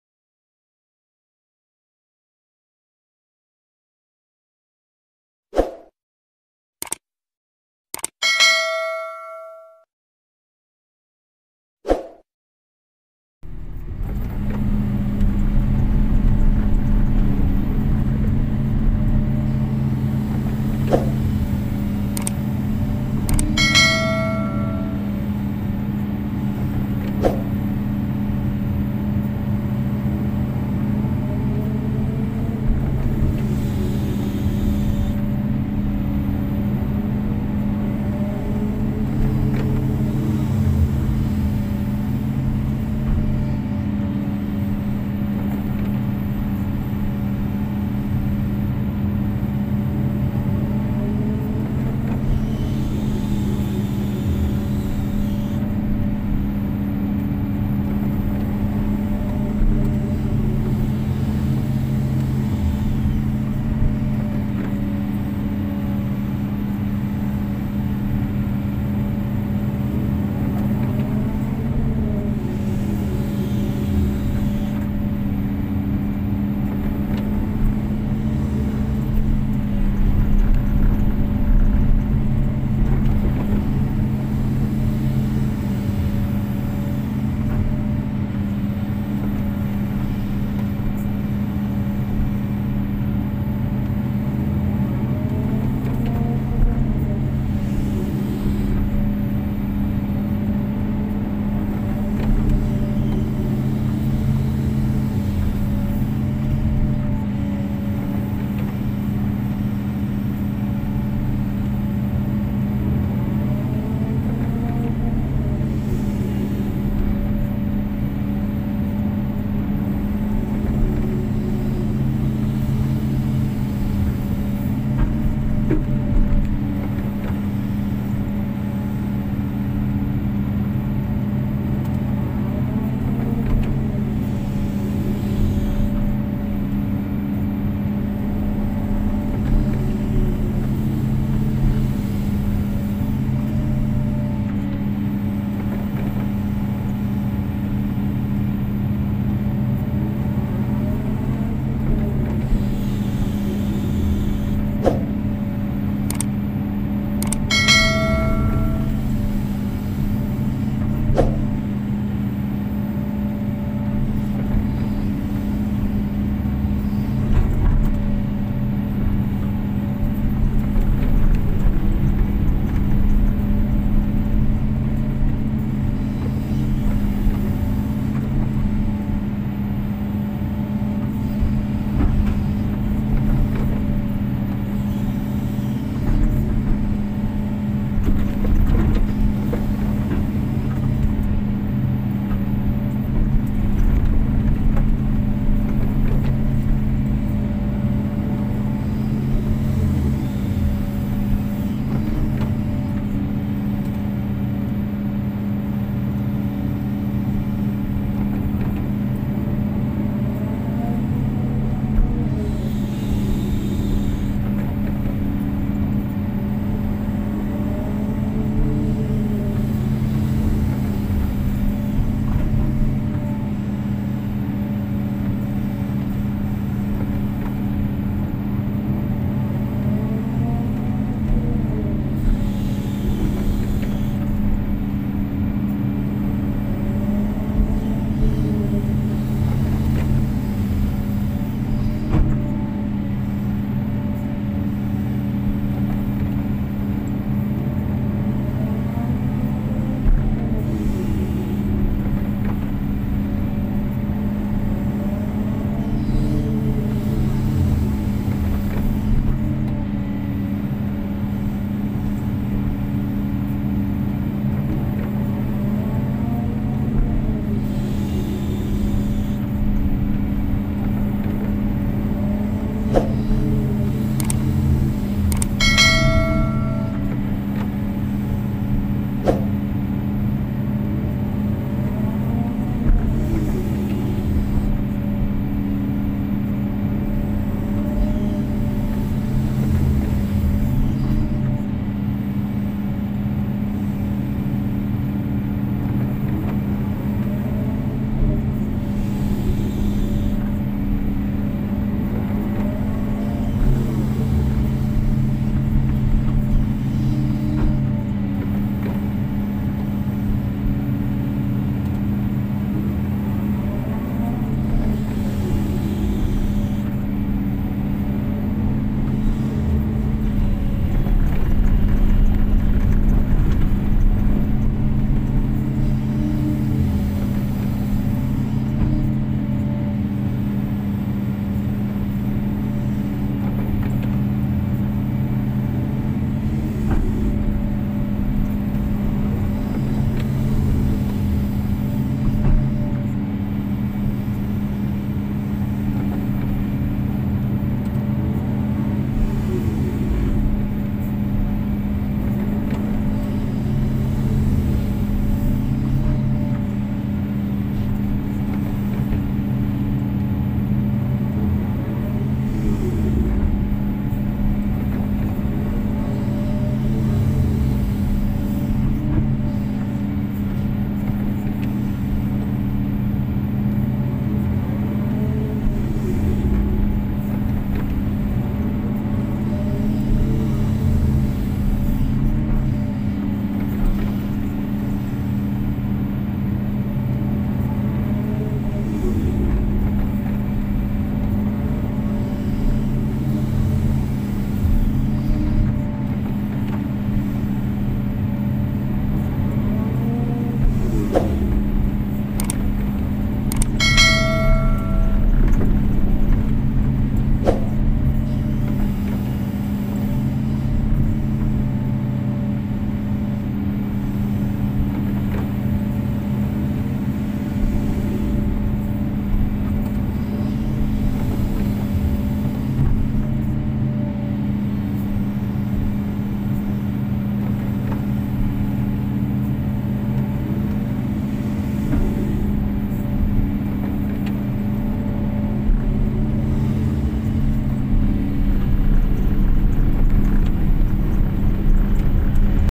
vlog 68 EXCAVATOR KOBELCO SK250-8KERUK ALIRAN AIR EFEK MUSIM HUJAN🌧️🌧️🌧️
Hello Temen2 Semua nya apakabar semoga sehat selalu dan dilancarkan rezekinya aamiin99
Hari saya bekerja keruk parit aliran air,banyak tanah yg turun
efek hujan terus-menerus.semoga lancar semuanya 🙏.

Thanks buat yang udah nonton, support terus ya chenel ini dengan cara